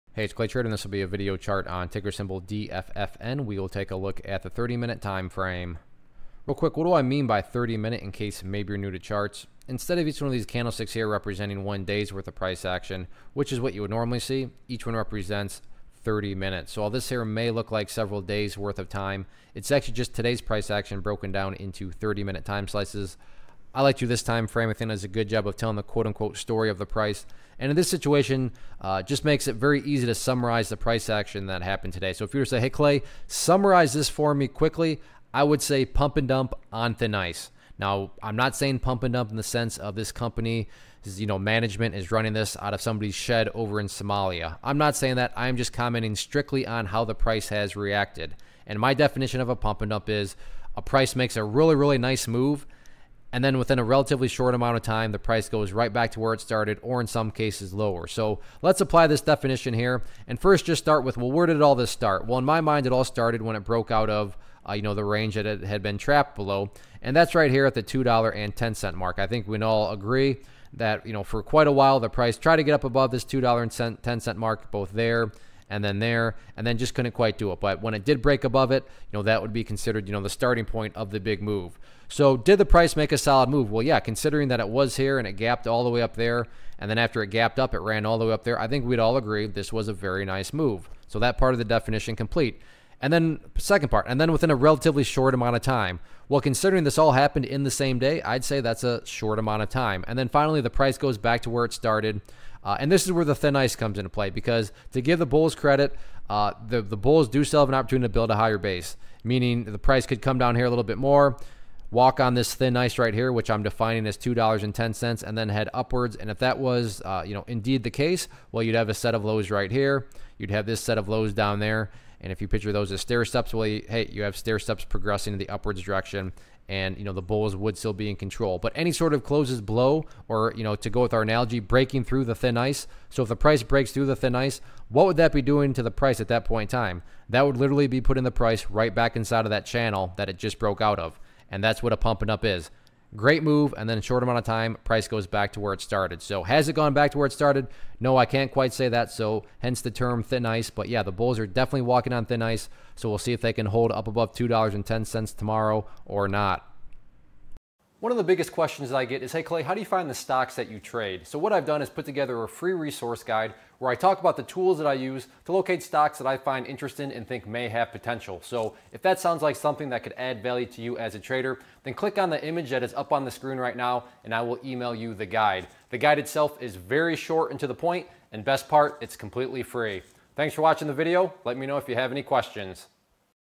Diffusion Pharmaceuticals Inc - DFFN Stock Chart Technical Analysis for 07-20-17
Learn how to read stock charts and identify technical patterns as ClayTrader does a quick stock chart review on Diffusion Pharmaceuticals Inc (DFFN).